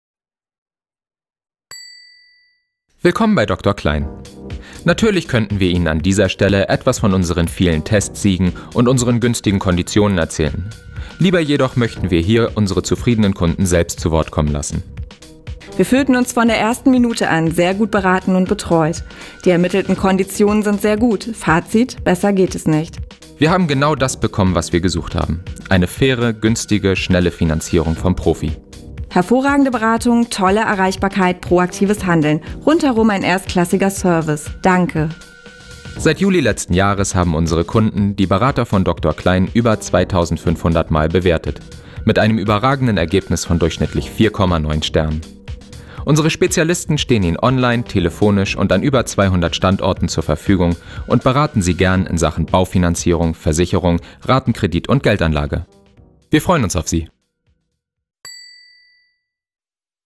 Dr. Klein: Hervorragende Kundenbewertungen und zahlreiche Testsiege
Dr. Klein überzeugt nicht nur mit zahlreichen Testsiegen und günstigen Konditionen − unsere Kunden sind außerdem sehr zufrieden mit unseren Beratungsleistungen. Seit Juli 2014 erzielten die Dr. Klein Berater mit 4,9 (von 5) Sternen überragende durchschnittliche Kundenbewertungen.